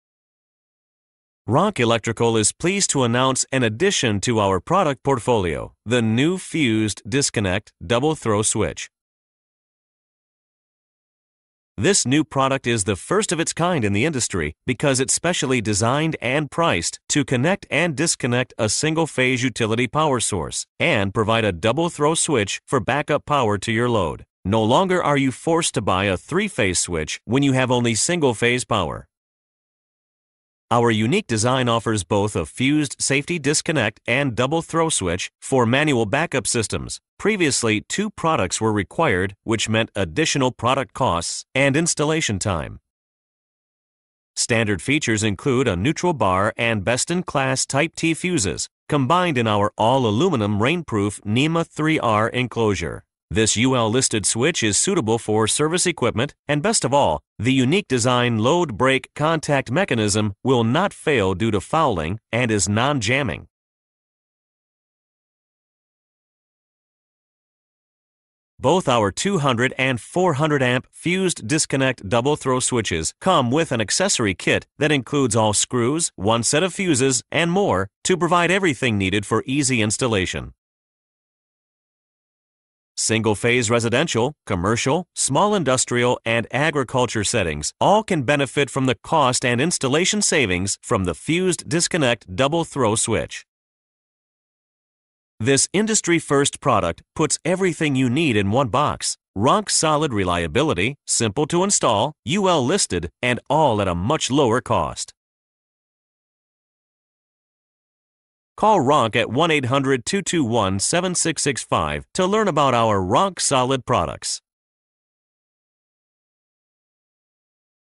Ronk Fused Disconnect Double-Throw Switch Product Introduction
The Fused Disconnect Double-Throw Switch is specially designed and priced to connect and disconnect a load from a single-phase utility power source and switch to backup power. No longer are you forced to buy a three-phase switch when you have only single-phase power.

This industry-first product puts everything you need in one box: “Made in the USA” Ronk reliability, simplicity, economy, and safety, all packaged in an all-aluminum, rainproof NEMA 3R enclosure.

Best of all, its unique design load-break contact mechanism will not fail due to fouling, is non-jamming, includes a neutral bar and uses best-in-class type “T” fusing.